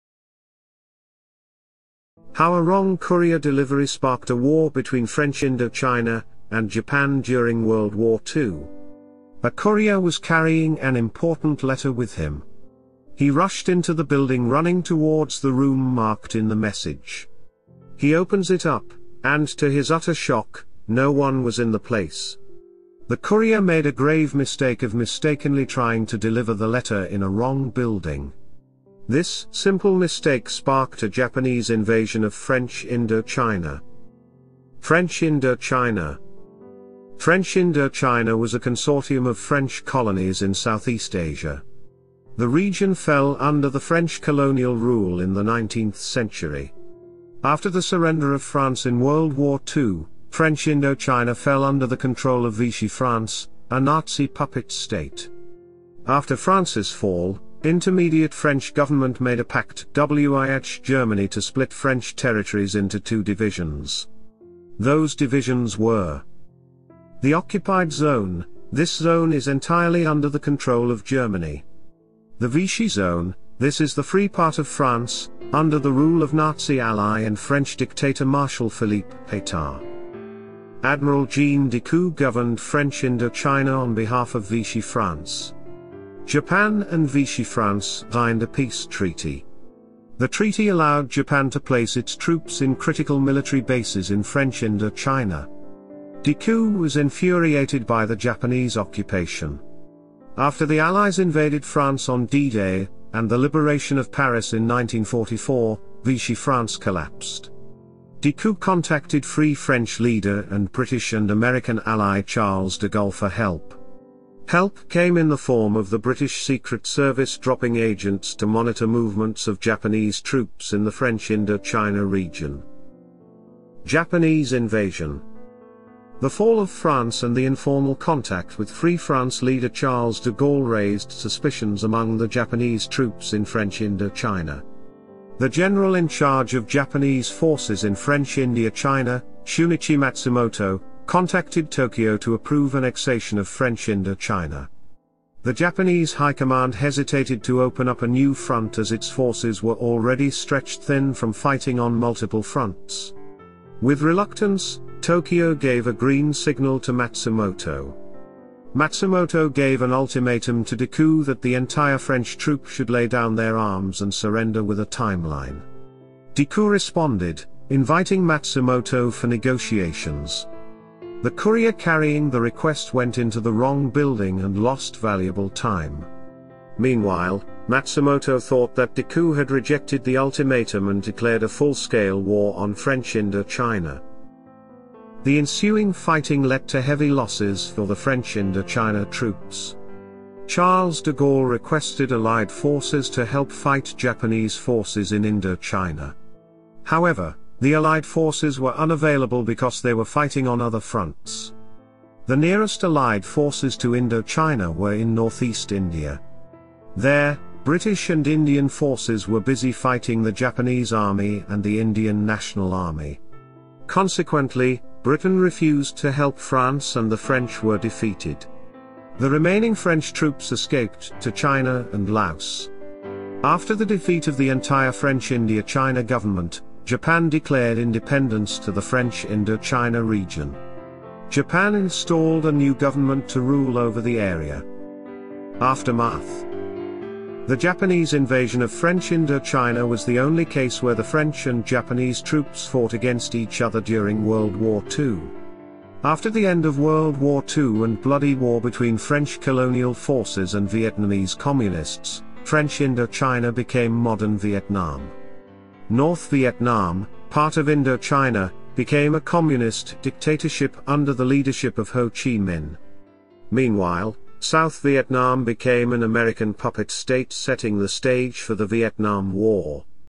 How A Wrong Courier Delivery Sparked A War
A courier was carrying an important letter with him. He rushed into the building running towards the room marked in the message. He opens it up, and to his utter shock, no one was in the place.
The courier made a grave mistake of mistakenly trying to deliver the letter in a wrong building. This simple mistake sparked a Japanese invasion of French Indo-China.